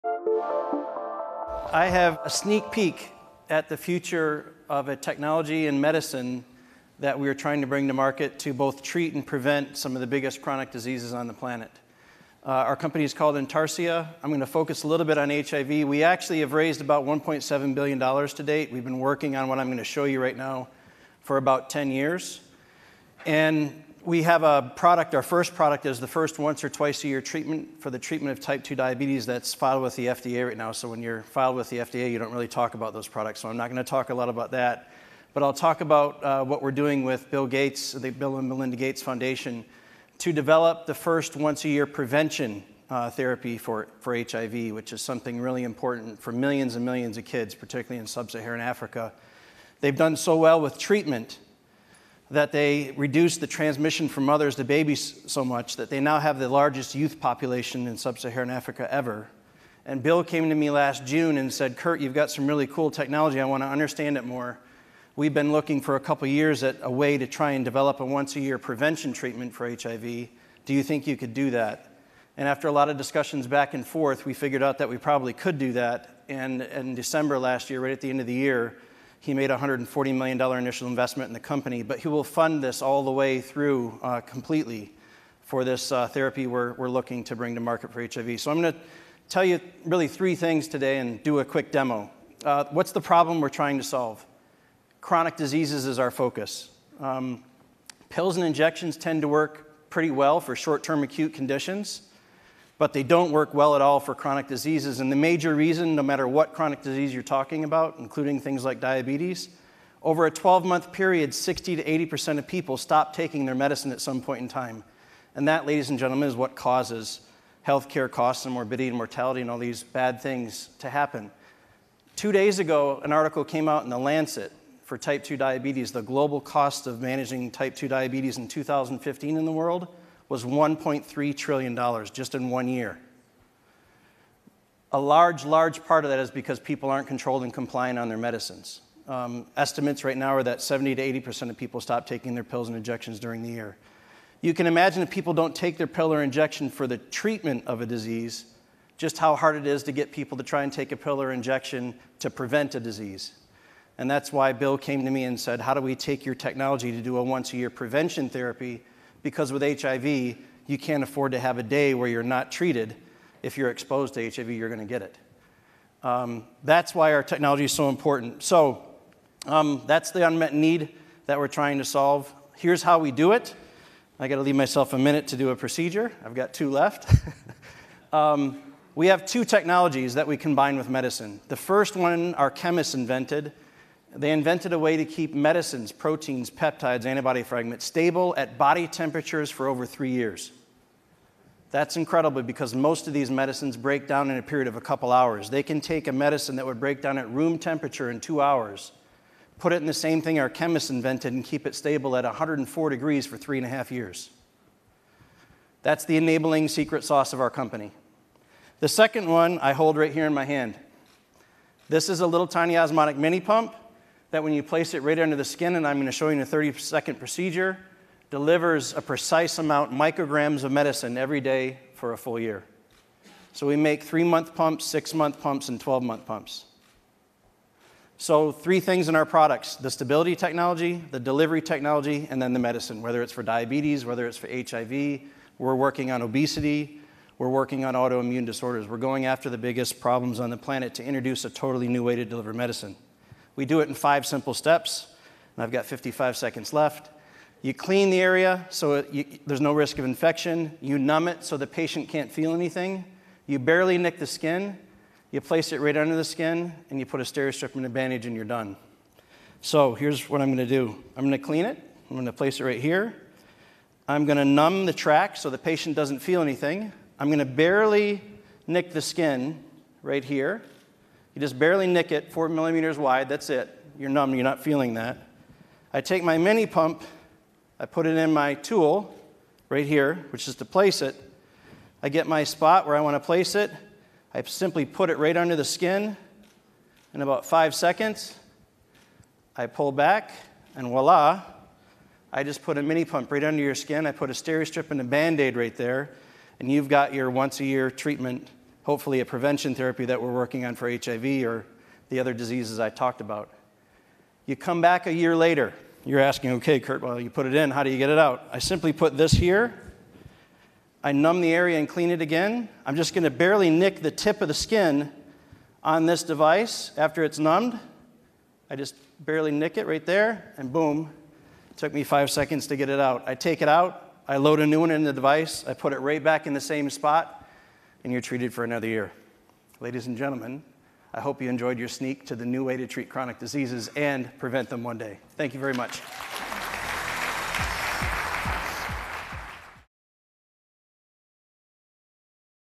The Implantable Drug That Can Prevent HIV | Brainstorm Health I Fortune
An up-close look at a new way of taking medicine: a matchstick-size device that when implanted under the skin releases a steady, continuous, and low-dose stream of medicine. Intarcia Therapeutics Inc. is now focused on delivering a drug for diabetes, but the possibilities don’t end there. Could this approach be used to stave off HIV, malaria and other infections?

-Kurt Graves, Chairman, President, and CEO, Intarcia